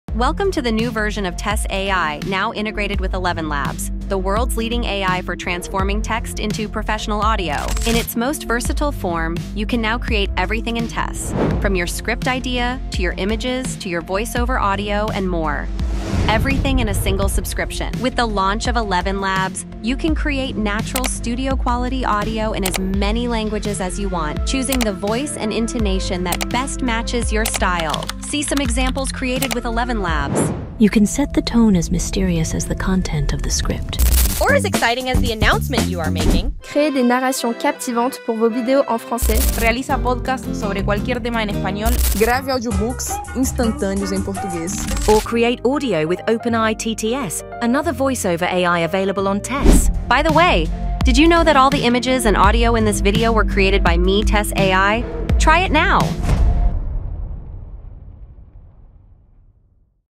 🎙️ ElevenLabs: IA Mais Avançada para Narrações Profissionais | Tess AI 4.1 | TTS Text-to-Speech
🇧🇷 | Tess AI 4.1: AI Voiceover com ElevenLabs! ✨

Transforme seus textos em áudios profissionais com apenas um clique. Com o AI Voiceover, criar narrações de alta qualidade nunca foi tão fácil. 🚀

Com poucos cliques:
🎧 Escolha entre ElevenLabs e OpenAI TTS
🌎 Alcance novas audiências com múltiplos idiomas
🎙️ Produza Áudios com qualidade de estúdio

Com a Tess AI 4.1 você vai além do roteiro e da imagem, você faz tudo em um só lugar.

Acesse agora e experimente o AI Voiceover na Tess AI! ➡️

🇺🇸 | Tess AI 4.1: AI Voiceover with ElevenLabs! ✨

Transform your texts into professional audio with just one click. Creating high-quality voiceovers has never been easier with AI Voiceover. 🚀

With just a few clicks:
🎧 Choose between ElevenLabs and OpenAI TTS
🌎 Reach new audiences with multiple languages
🎙️ Produce studio-quality audio

With Tess AI 4.1, you go beyond script and image, you do everything in one place.

Access now and try AI Voiceover on Tess AI! ➡️